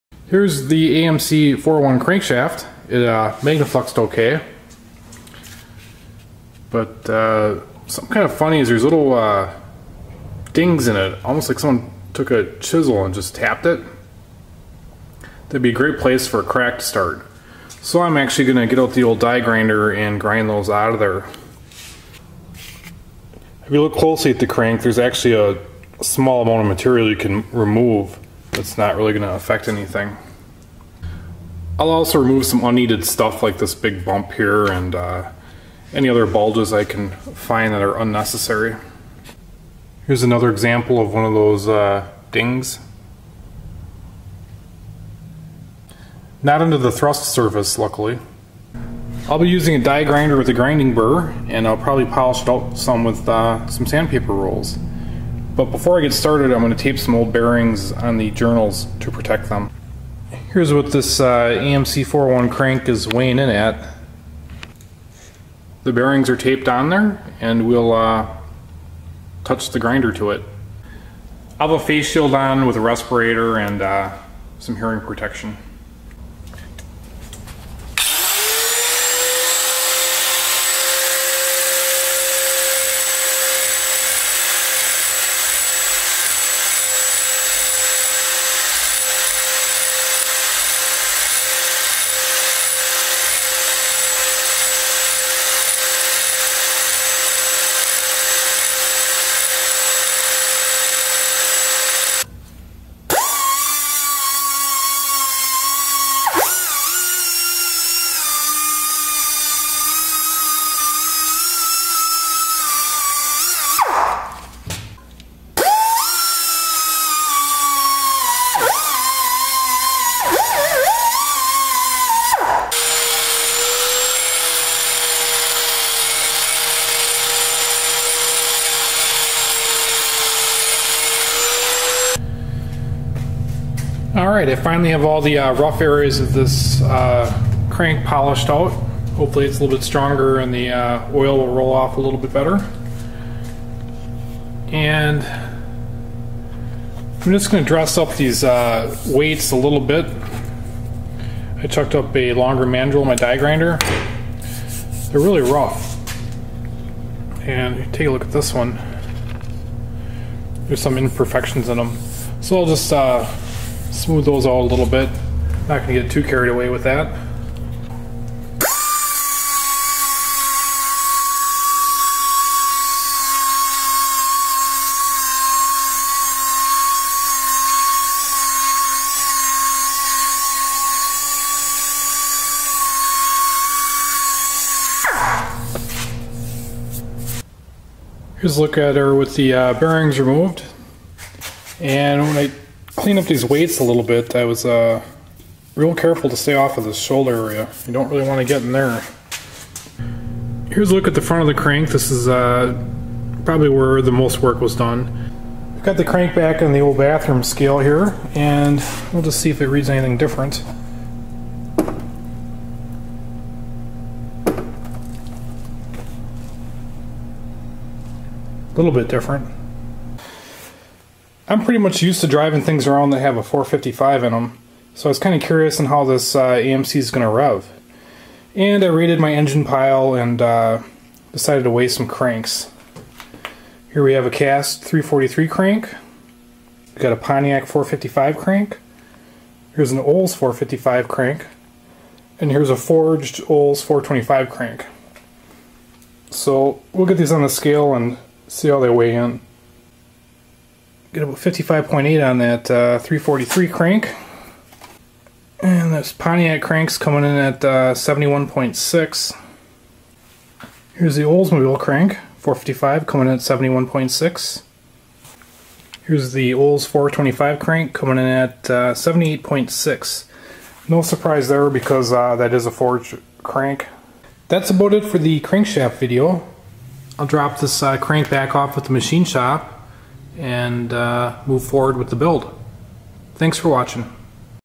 Crankshaft Flash and Nick Removal
Cleaning up the AMC 401 Crankshaft before getting the rotating assembly re-balanced.  This crank and some others are weighed in the end of the video.